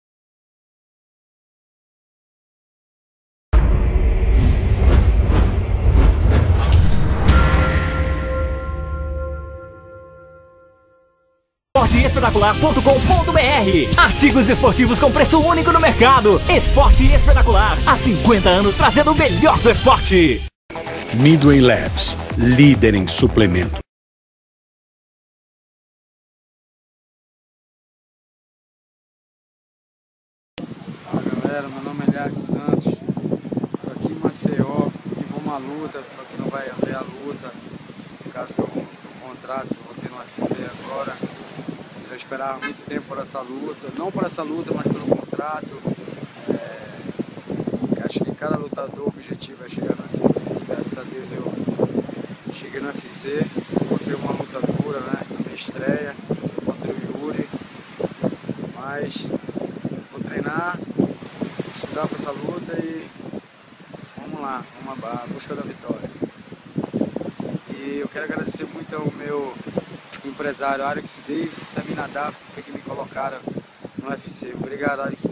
Iliarde Santos fala sobre sua estréia no UFC em Jaraguá do Sul
Atleta paraense da Fight Agency, Iliarde Santos  fala sobre sua estréia no UFC no Combate 2 dia 18 de Maio em Jaraguá do Sul contra o tambem atleta paraense Iuri Marajó.